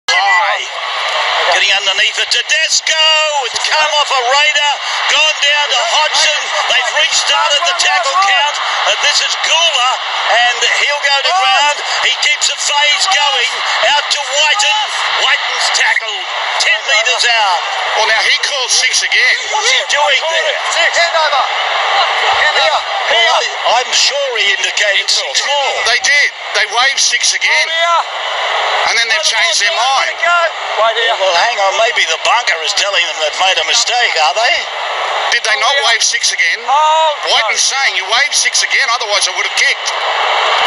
Nrl grand final 6 again controversy. 2019 Nrl grand final Canberra Riders vs Sydney Roosters
Referee makes the biggest stuffup in NRL history by reversing a 6 again call. The roosters immediately score after.

Worst call in Rugby League history.

Im not a raiders fan and this was the worst called game ive seen.